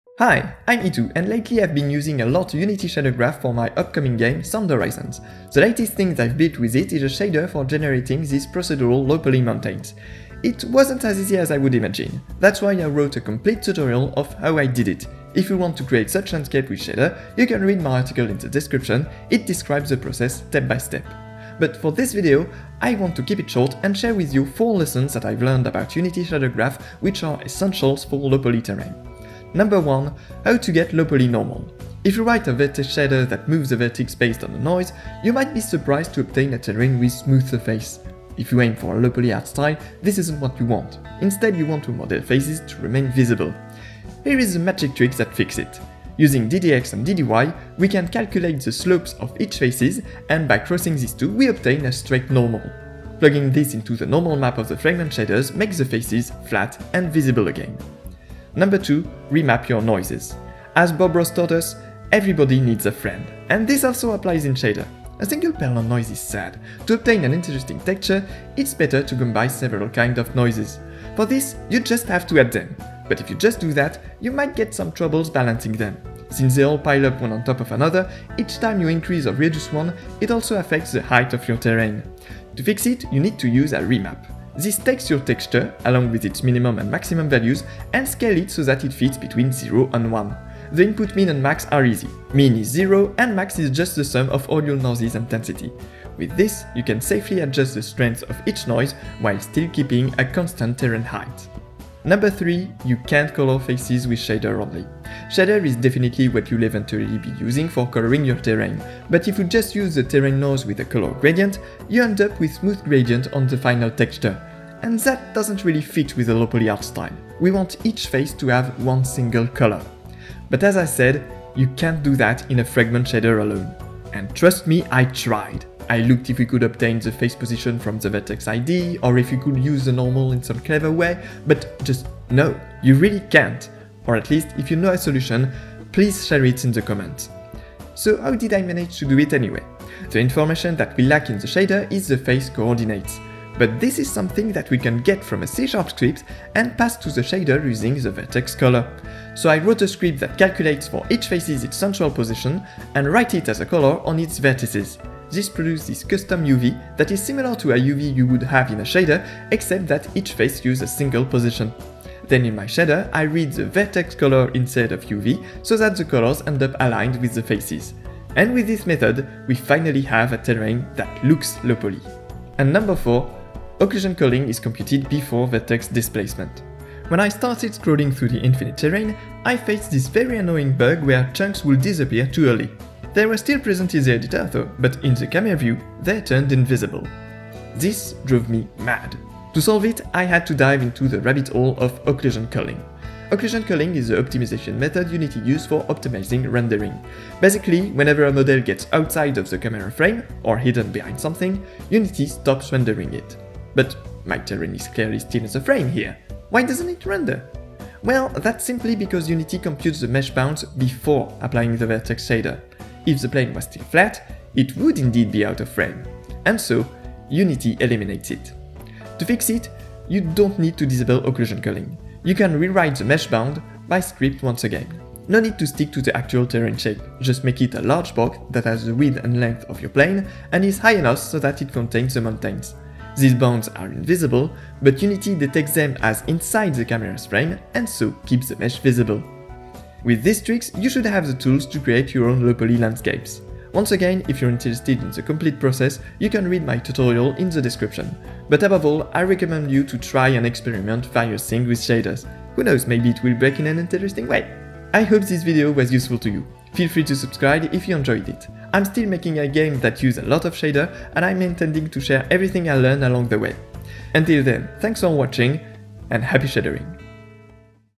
4 Unity Shader Graph tips for low-poly terrain (EN)
After generating a procedural low-poly landscape with Unity Shader Graph, I wanted to share some of the lessons I learned. While shaders are great tools to model terrain, there are some pitfalls that are useful to know!

This video focus on 4 specific techniques, to help you create your own procedural landscape. However, I also wrote a complete tutorial of my method, that could also help you:

Other resources:

Unity3D Procedural Low-poly terrain, by Evgeniya Glazycheva : Although this method relies on script rather than shader, I sill used its mesh generation part

Derivatives DDX DDY, by Ben Cloward: If you want to know more about DDX and DDY